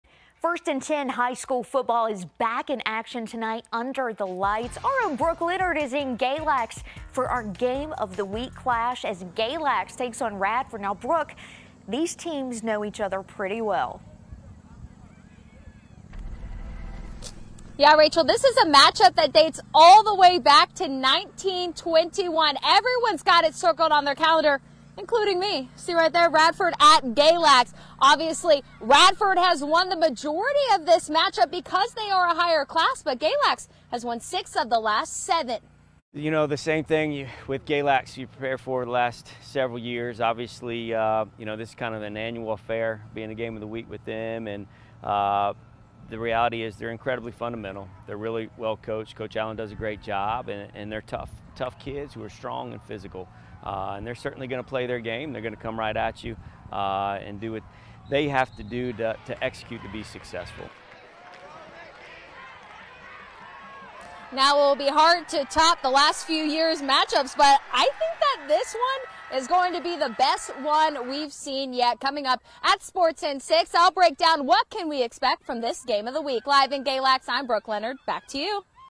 Game of the Week: Radford faces off against Galax
The Radford and Galax football matchup dates all the way back to 1921